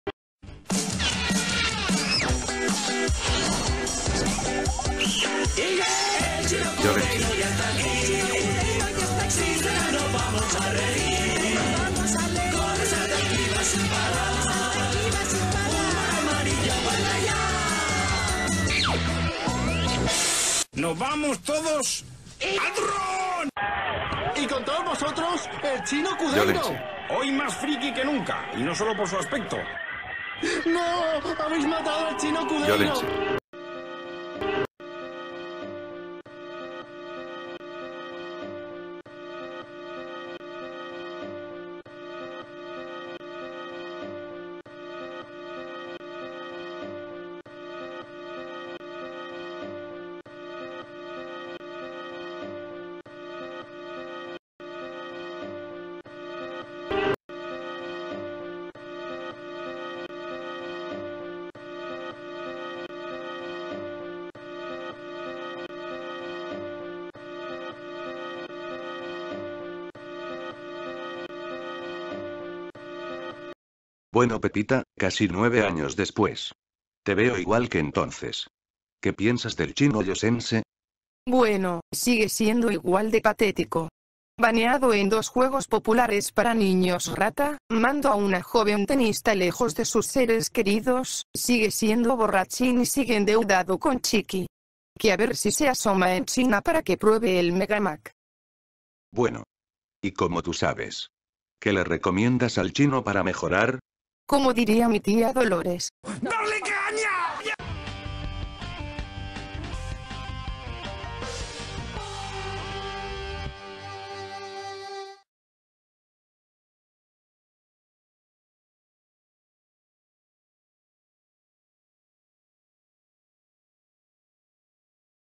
Montajes JC:El Chino Llosense V2(Remake 2019)
Remake y version actualizada de un video que hice en 2010 sobre un chino llosero(ficticio,no existe) que a lo chino Cudeiro salen epitafios con datos de algunos lloseros y lloseras(alguna real pero la mayoria ficticias)